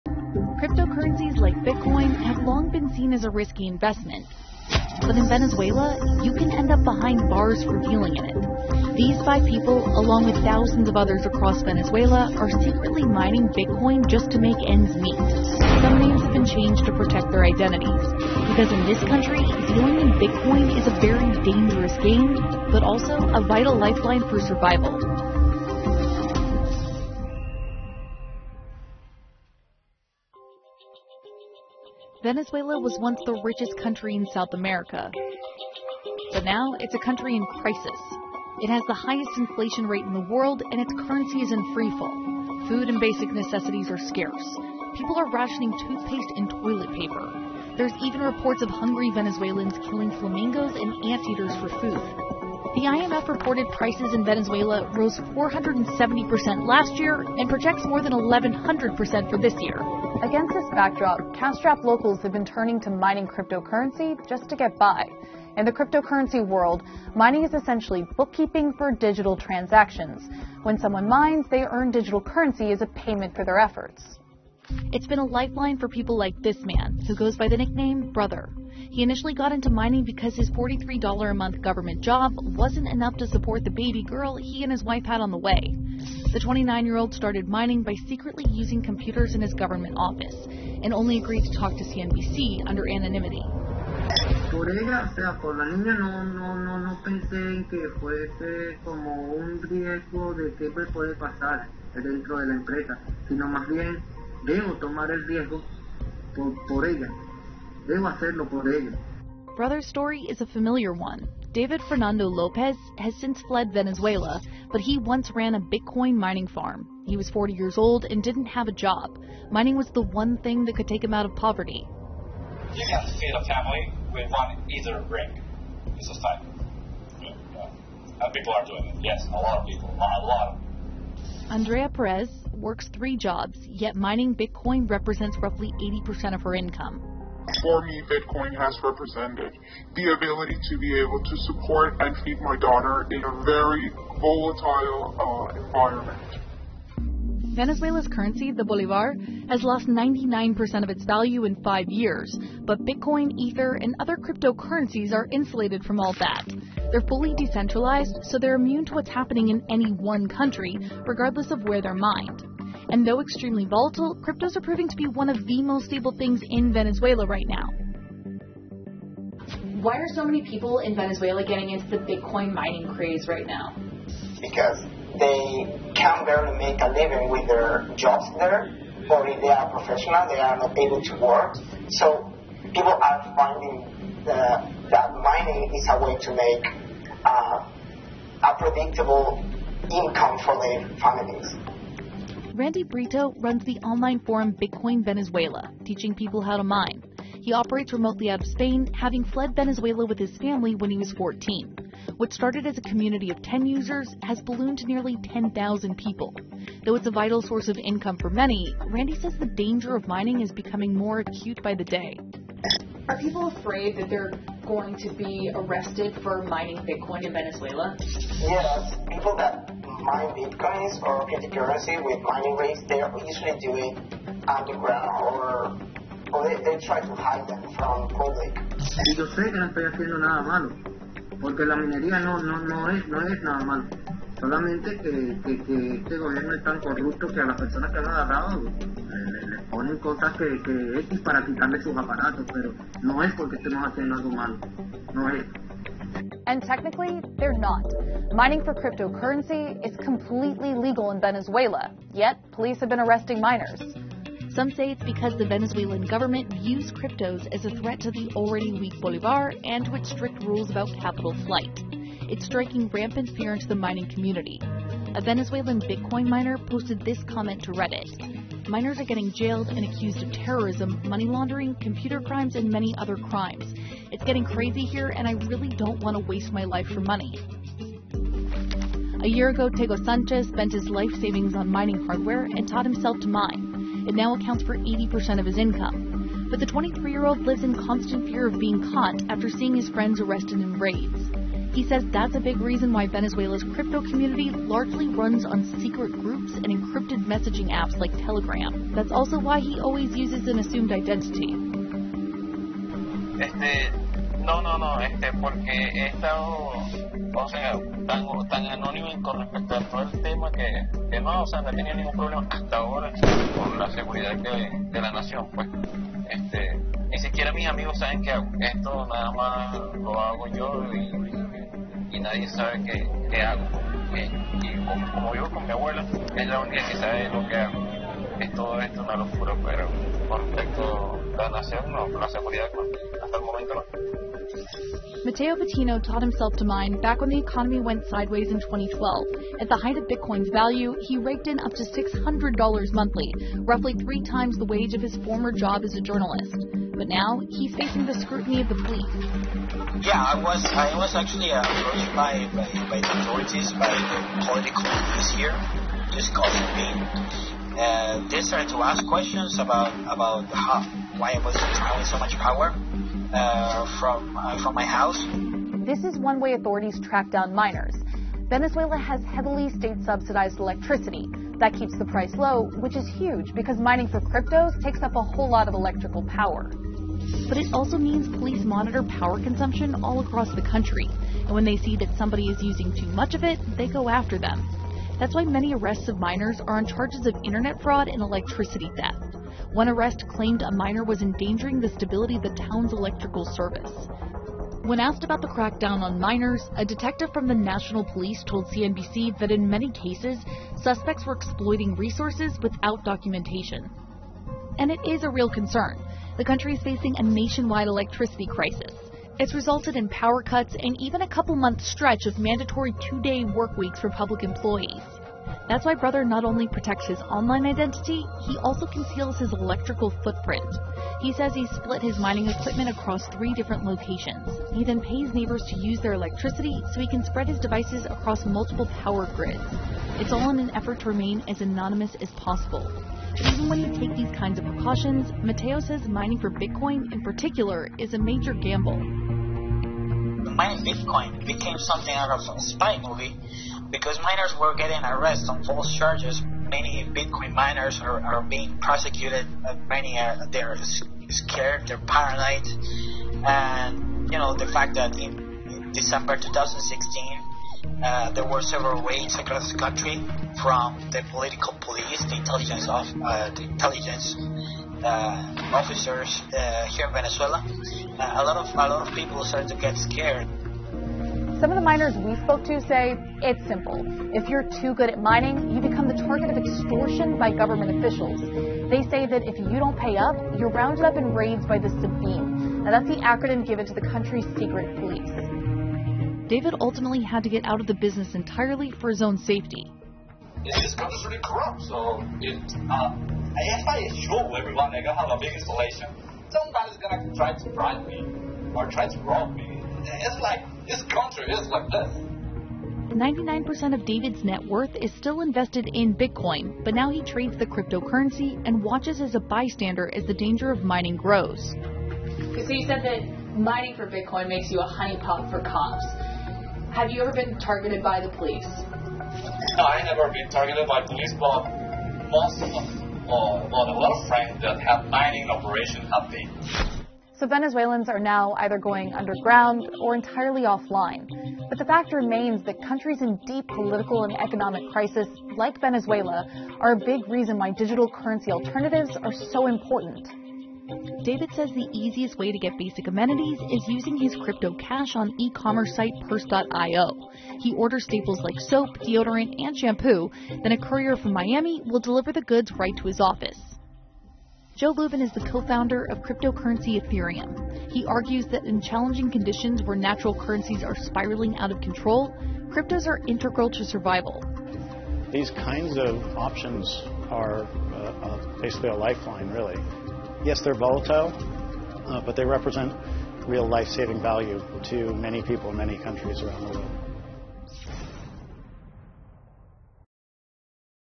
bitcoin mining is dangerous in these country.
list of countries where bitcoin mining is just illegal.